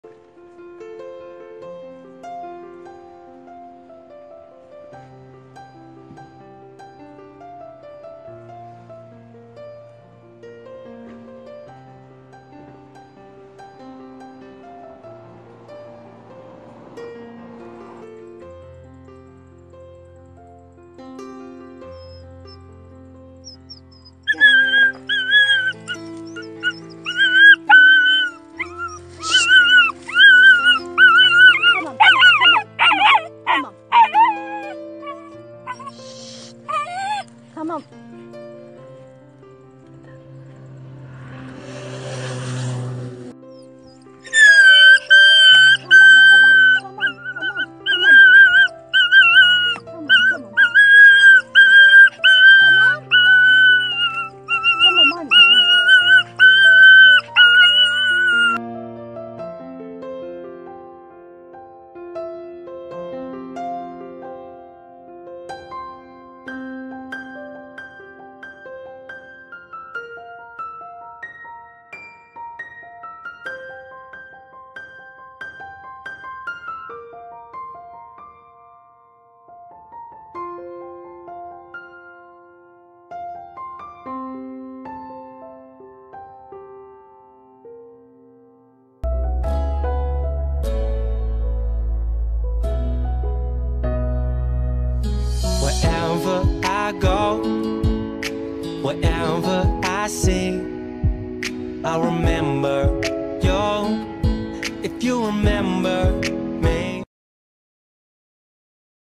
Rescue Homeless Dog with Broken 2 Legs Found In The Hills
@Credit: sbht_lovesdogs

@You can donate to help "sbht_lovesdogs" create a safer world for animals via and you can inquire about adopting